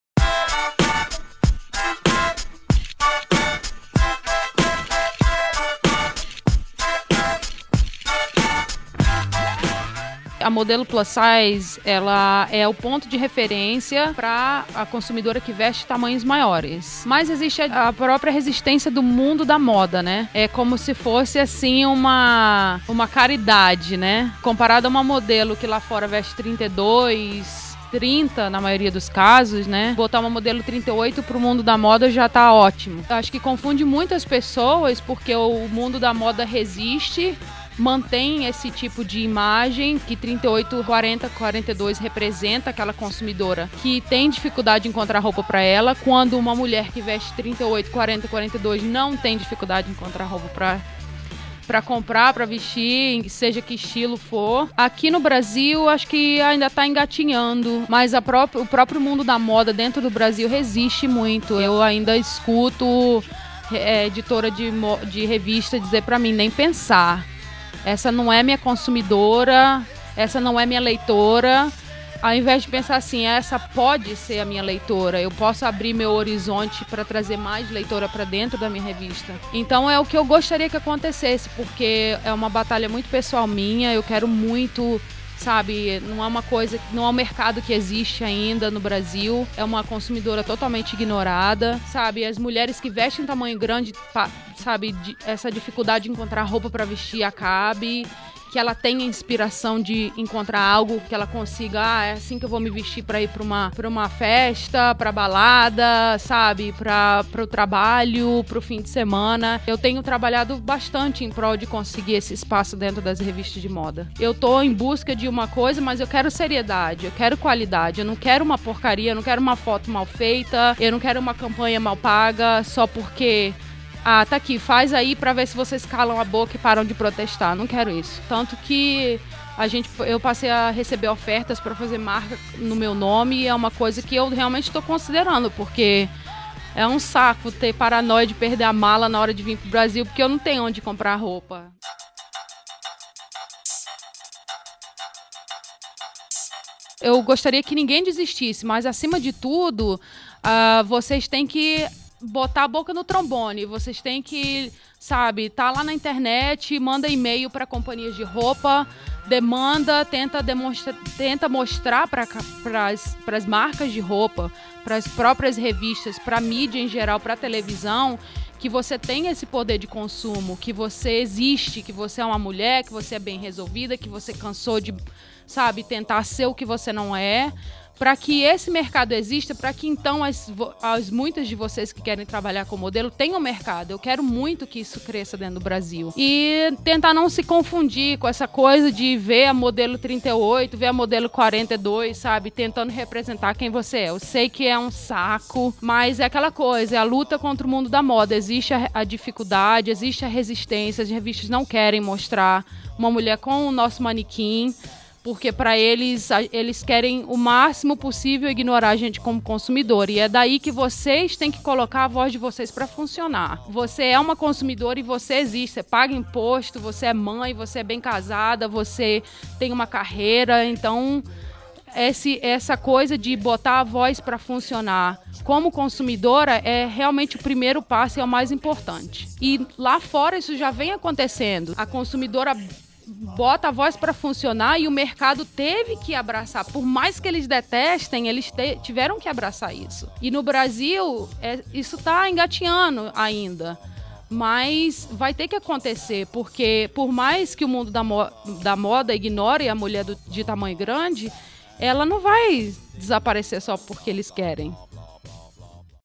Modelo Plus size Fluvia Lacerda - Lifestyle - Virgula
"As pessoas não se conformam que é possível ser gordinha e sexy", diz a Top Fluvia Lacerda na sua entrevista para o site Virgula.